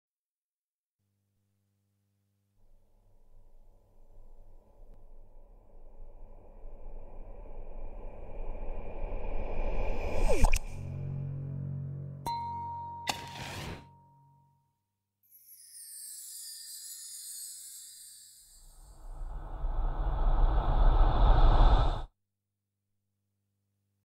Dolby Digital Surround EX: Waterfall (Center Channel)
Source used: An ISO of "Explore Our World" (2002 Dolby Digital Demo DVD)